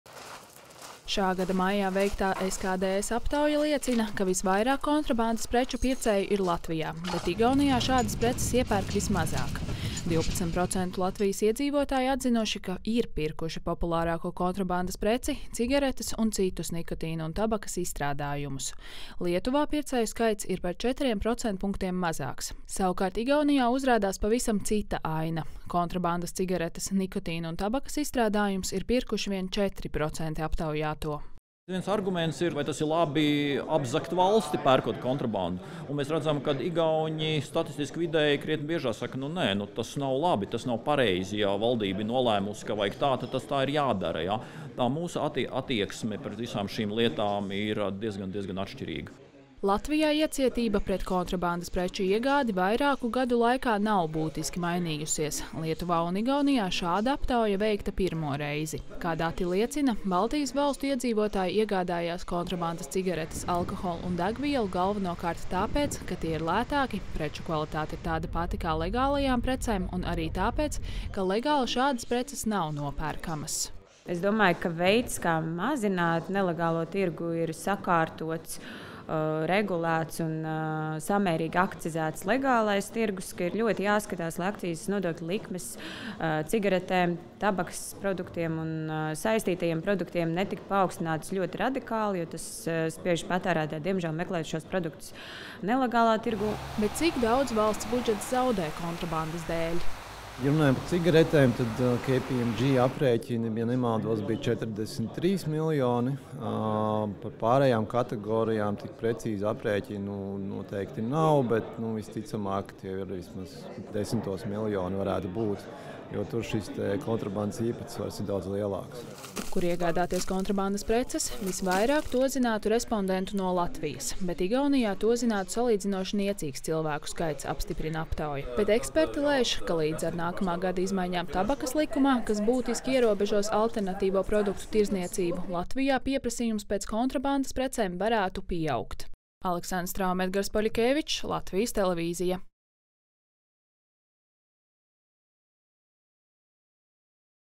SKDS: Igaunijā vismazāk pērk kontrabandas preces
Par kontrabandas preču iegādi sabiedrība Latvijā un Lietuvā ir daudz iecietīgāka nekā Igaunijā. Tieši igauņi uz Baltijas fona krasi atšķiras. To apstiprina SKDS veiktais pētījums, kurā Baltijas iedzīvotāji aptaujāti par kontrabandas preču iegādi. Latvijā šādi pētījumi tiek veikti jau vairākus gadus, taču pārējās Baltijas valstīs iedzīvotāji tika aptaujāti pirmo reizi.